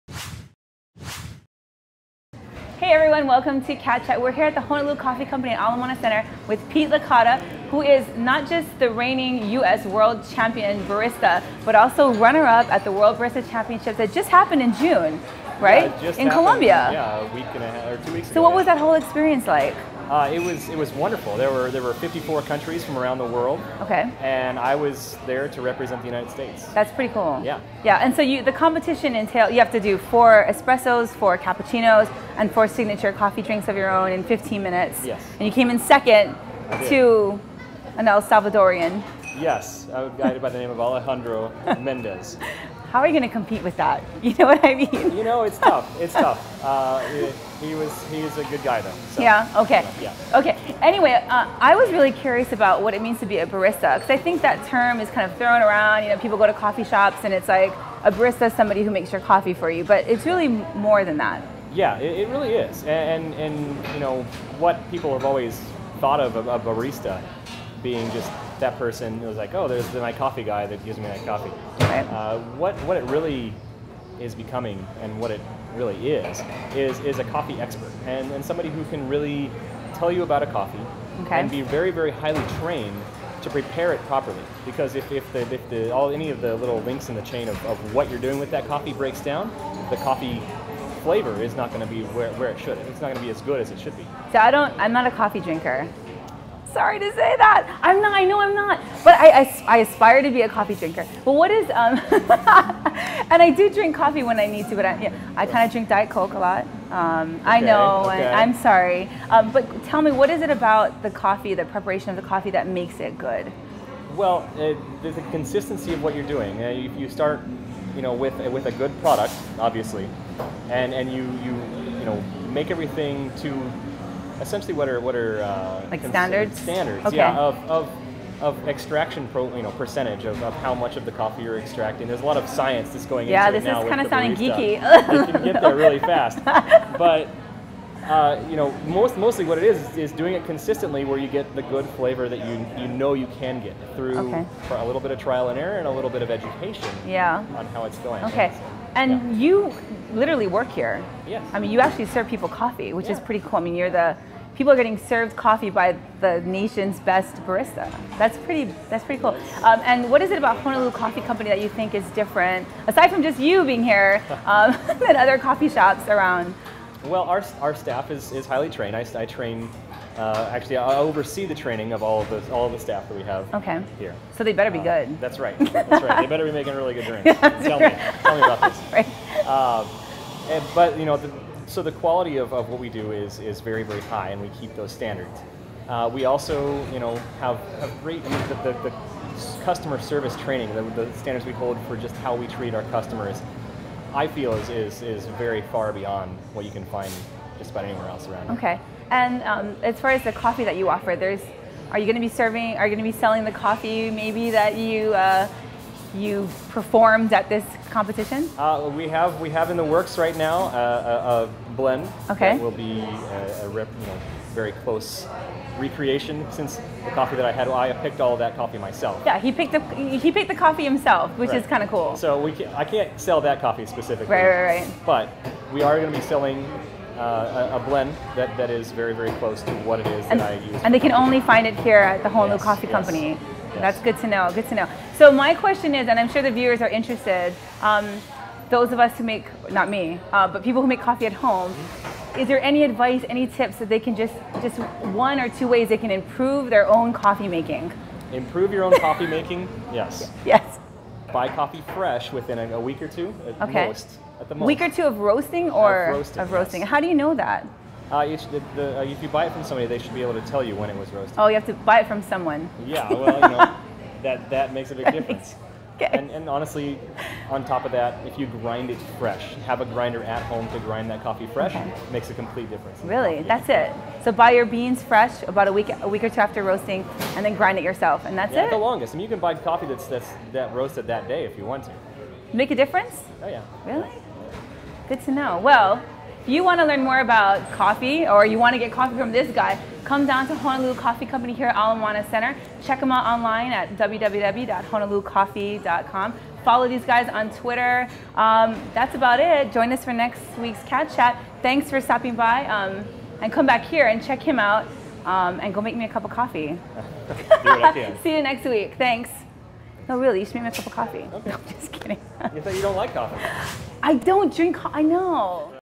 Cat Chat 22: Coffee by a champ - Nonstop Honolulu
It's not everyday you get your cup of coffee made by the country's best barista.

Really, Pete Licata, the coffee quality manager and barista trainer at the Honolulu Coffee Co. at Ala Moana Center — and reigning U.S. barista champ who placed second in the world this month — only works five days a week.

Originally from Missouri — and a huge beer connoisseur — Licata earned the runner-up spot at the World Barista Championship held earlier this month in Bogota, Columbia. Competing as the U.S. champ, Licata progressed through the field of 54 baristas to reach the final round, pitted against five other finalists from Japan, Spain, Australia, El Salvador and the United Kingdom. They each prepared four espressos, four cappuccinos and four original signature coffee drinks to exacting standards in a 15-minute performance judged by an expert panel. Licata lost to Alejandro Mendez of El Salvador to become the first Hawaii barista to ever make it that far in a world competition.

And he'll make you a soy latte if you catch him on his shift.

We stopped by to chat with the second-best barista on the planet and got him to brew us some coffee, too.